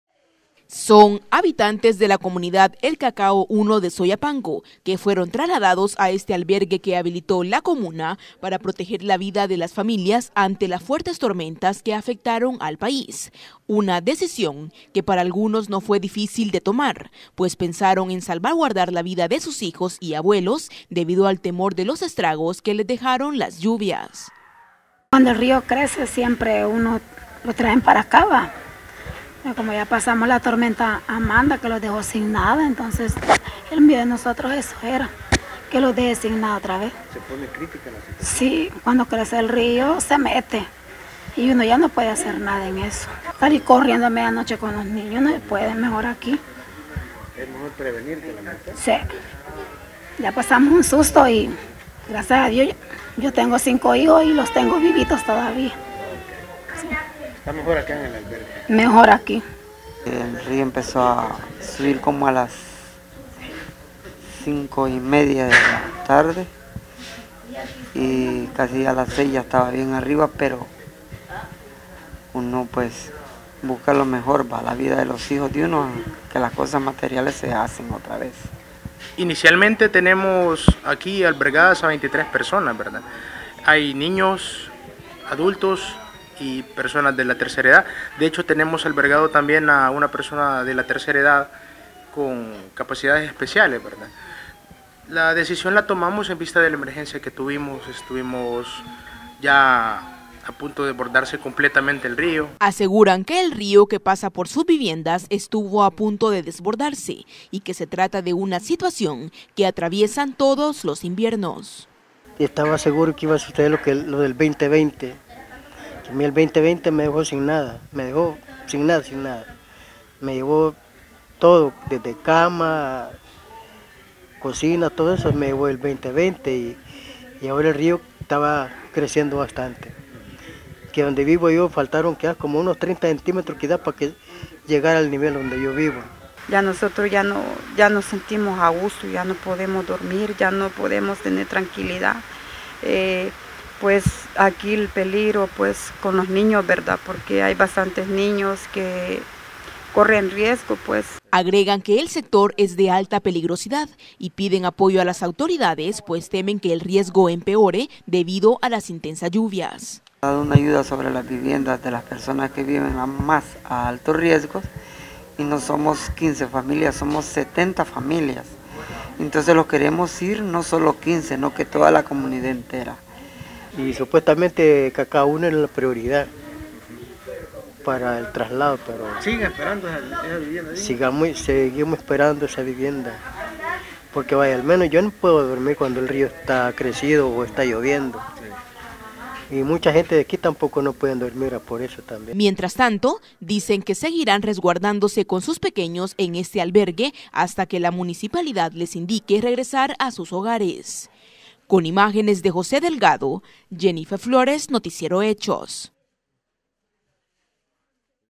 Evacuación de residentes de comunidad El Cacao 1 de Soyapango
Más familias fueron evacuadas de forma preventiva en el municipio de Soyapango, debido a la alta peligrosidad y el riesgo que existe en la zona donde habitan.